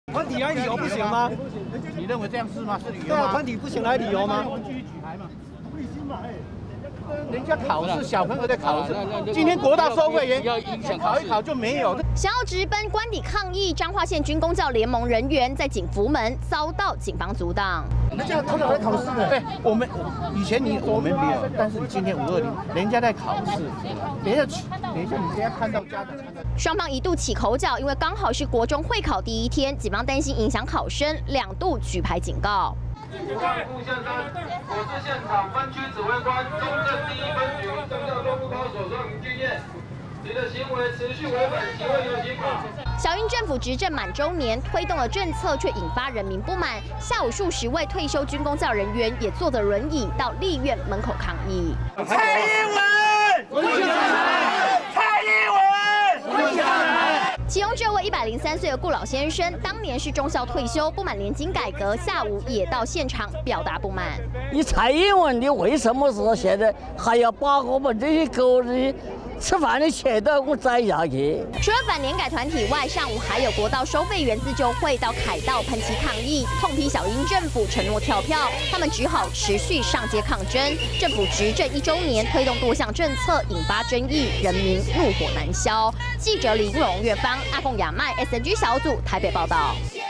20170520中天新聞 欲赴官邸抗議 反年改團體遭警阻擋
小英政府執政一週年，推動多項政策，引發人民不滿，上午國道收費員自救會，百位成員到凱道噴漆抗議，下午則有反年改團體，在立院門口表達不滿，還有軍公教聯盟人員，一度要直奔總統官邸抗議，途中遭到警方阻擋，雙方起口角。